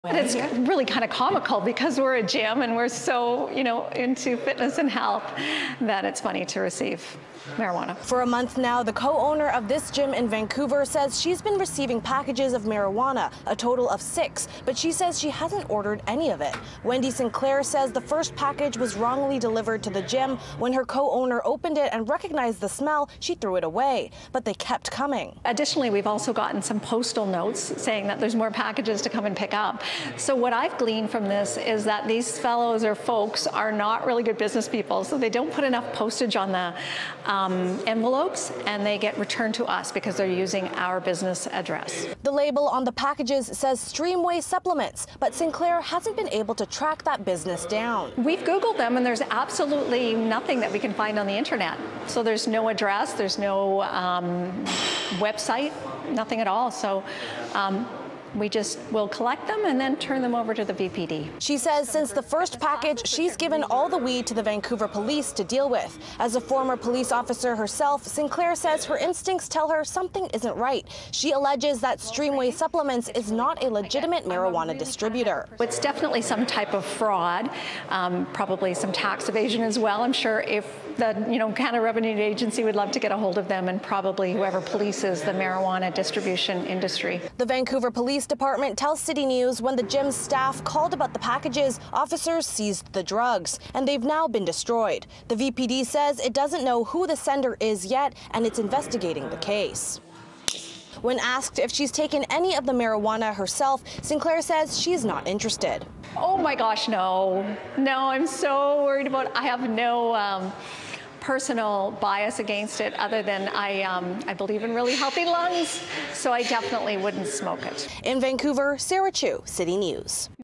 Vancouver gym owner keeps getting marijuana packages — but they aren't hers
For a month now, the co-owner of a gym in Vancouver says she’s been receiving packages of marijuana — a total of six — but she says she hasn’t ordered any of it.